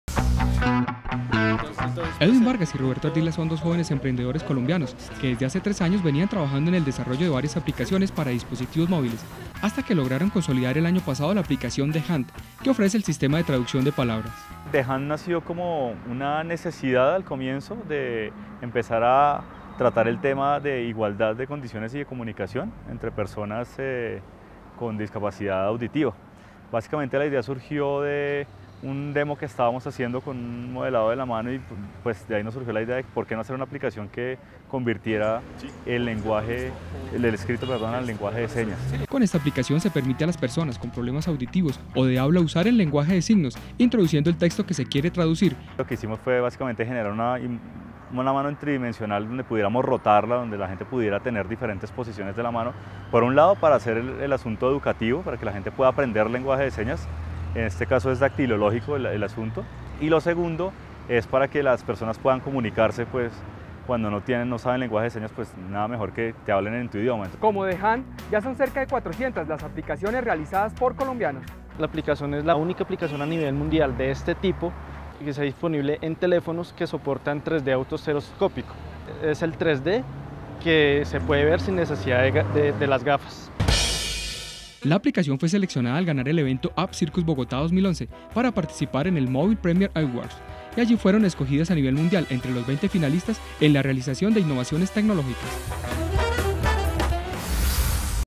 Aplicaciones colombianas
El tema de las aplicaciones  que permiten la inclusión social, veamos cómo  desde el celular ahora es posible comunicarse por lengua de señas  con personas con discapacidad auditiva.